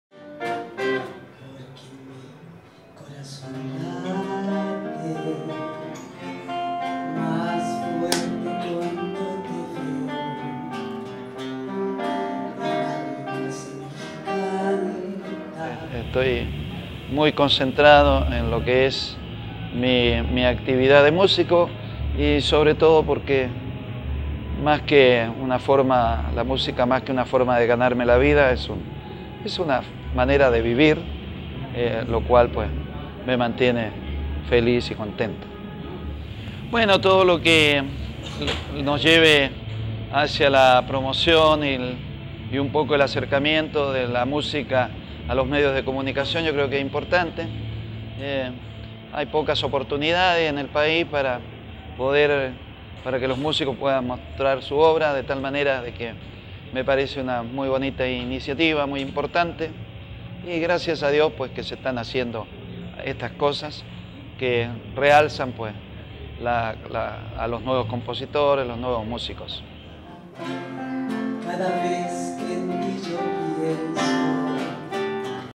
Cosecha Nacional @ Pulsar / Germaín de la Fuente
Germaín de la Fuente, voz de Los Ángeles Negros en Feria Pulsar 2013